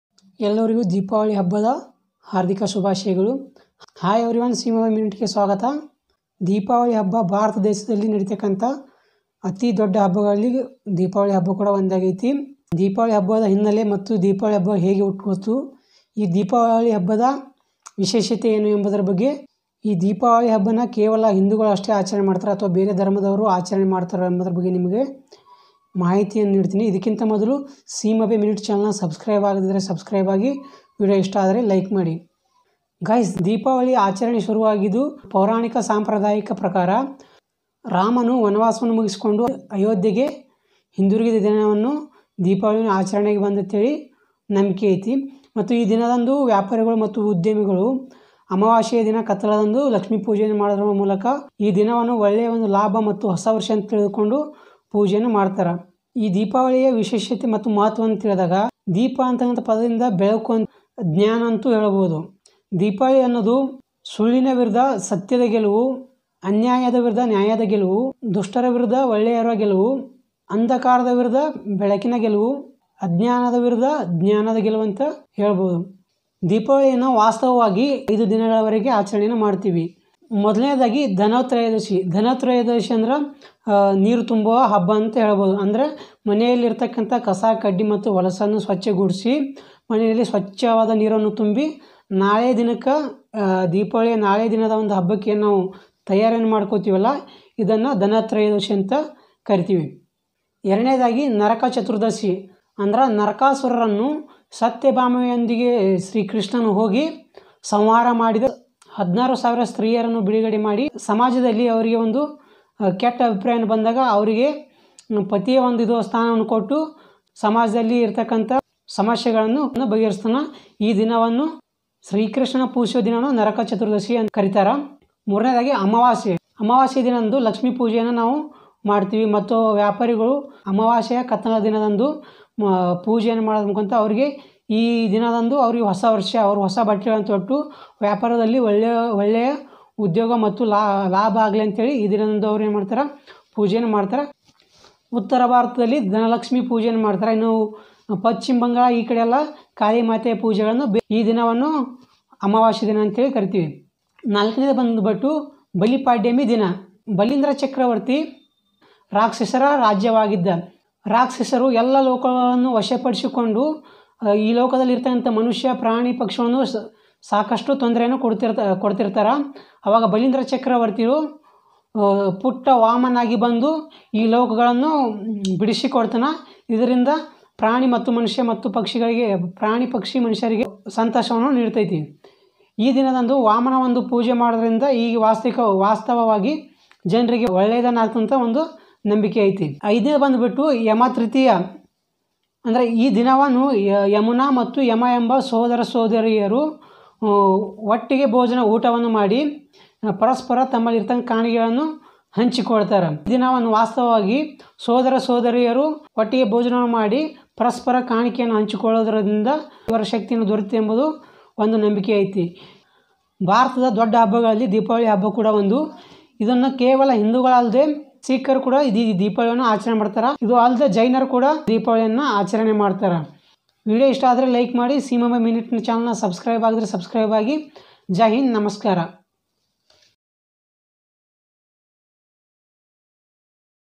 ದೀಪಾವಳಿಯ ವಿಶೇಷ ಏನು ಗೊತ್ತಾ? ದೀಪಾವಳಿ ಹಿಂದೂಗಳಷ್ಟೇ ಆಚರಣೆ ಮಾಡತಾರ? Simabe Minute
ದೀಪಾವಳಿಯ ವಿಶೇಷ ಏನು ಗೊತ್ತಾ? ಹಿಂದೂಗಳಷ್ಟೇ ಆಚರಣೆ ಮಾಡತಾರ?